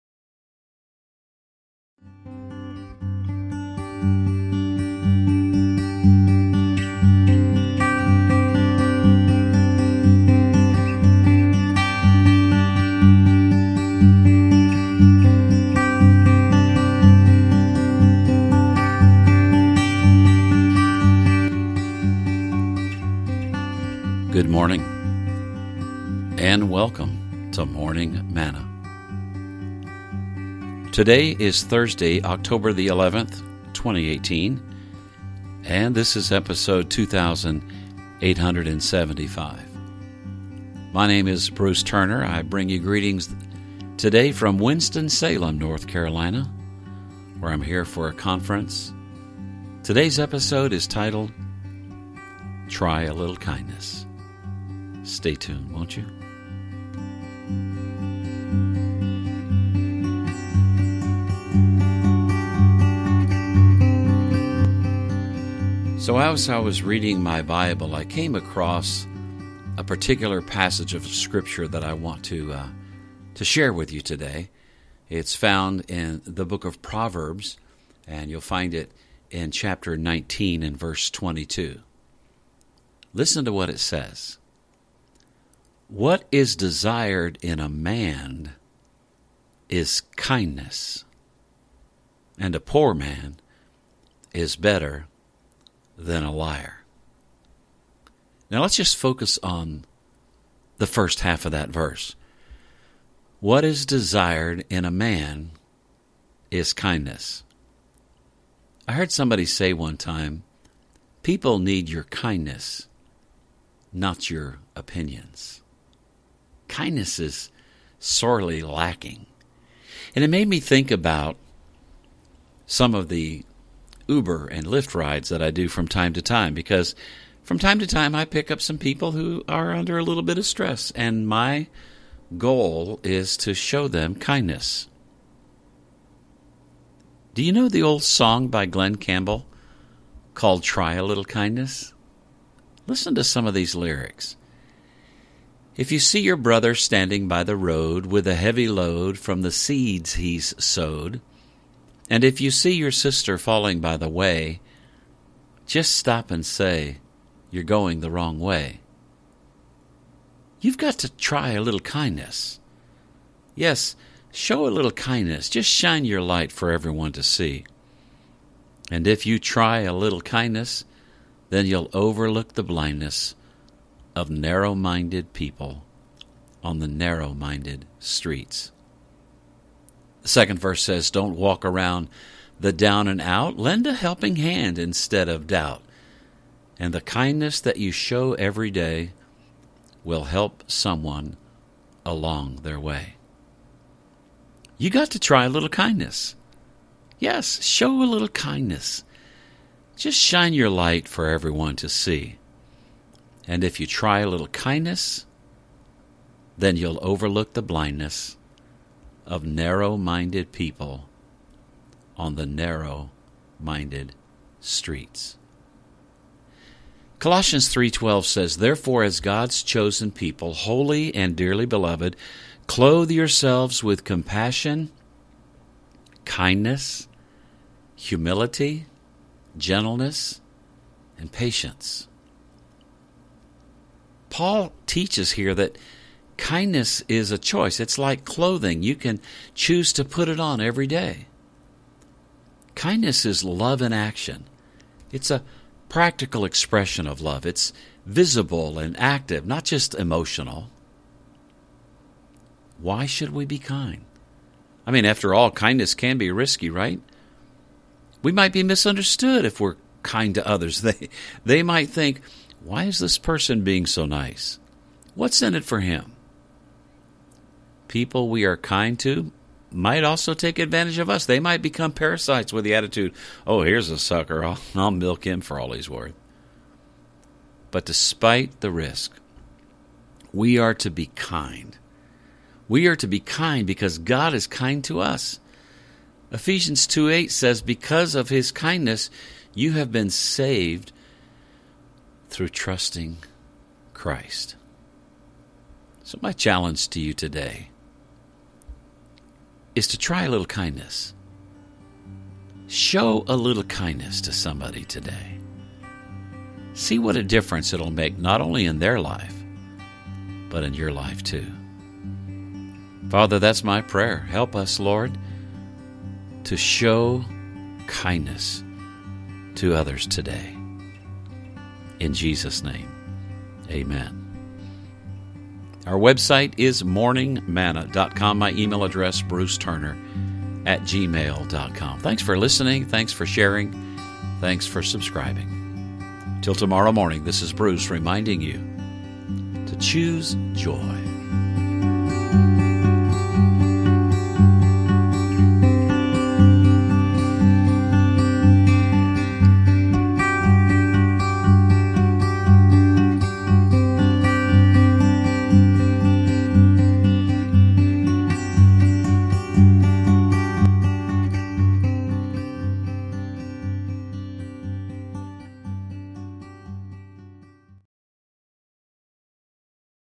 Try A Little Kindness - Morning Manna #2875
Someone has said, people need your kindness, not your opinions. We live in a very opinionated world. Proverbs 19:22 tells us; "What is desired in a man is kindness...". My challenge to you today is to try a little kindness in all your relationships today and see what a difference that will make.